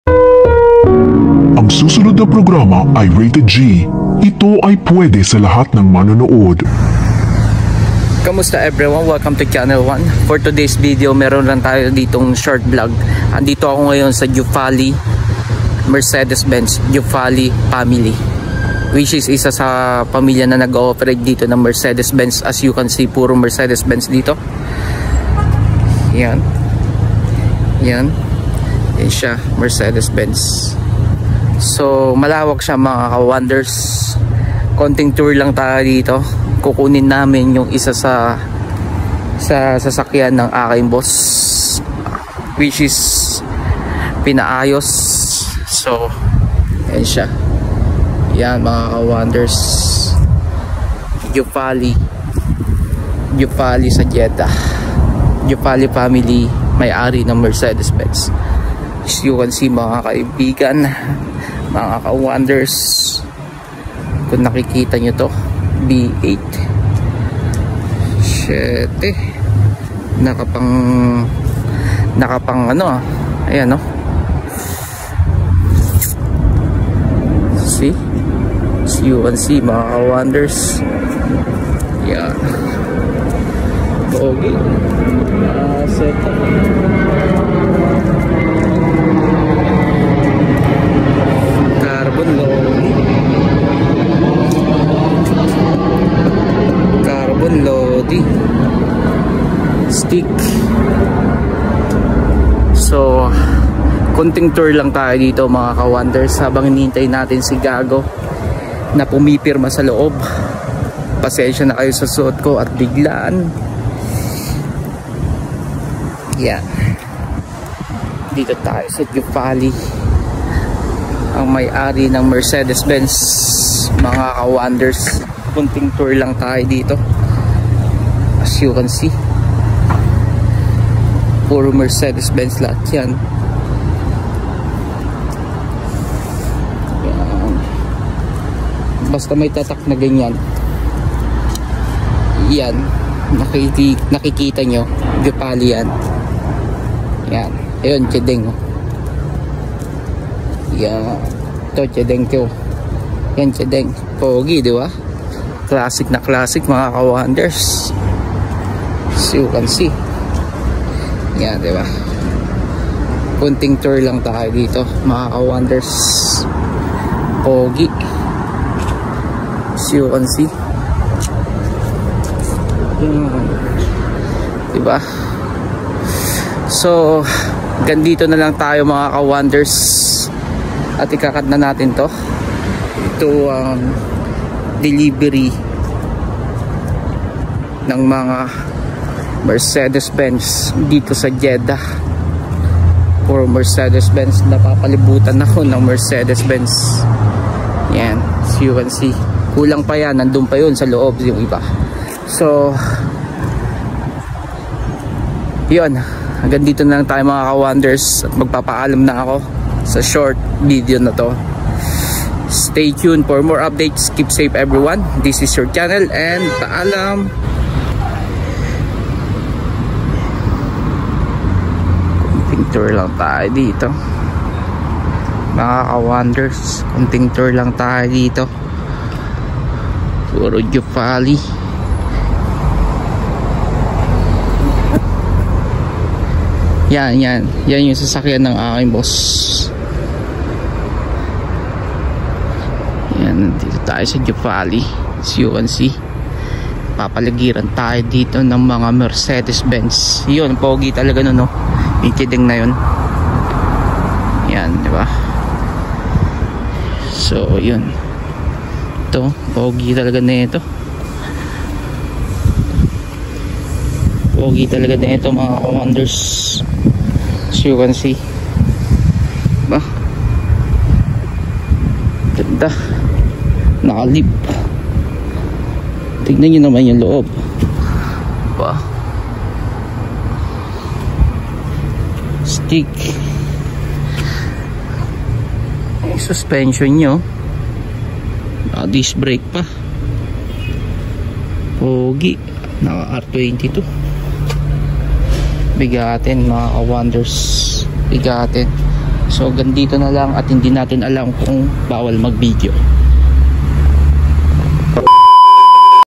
Quick tour @ mercedes benz showroom by juffali co
Mercedes benz jeddah owner by juffali group of co grabe daming benz hehe lalo na mga lifted sila lang namn ang may ari ng benz dito sa buong jeddah ksa lakas dba 👌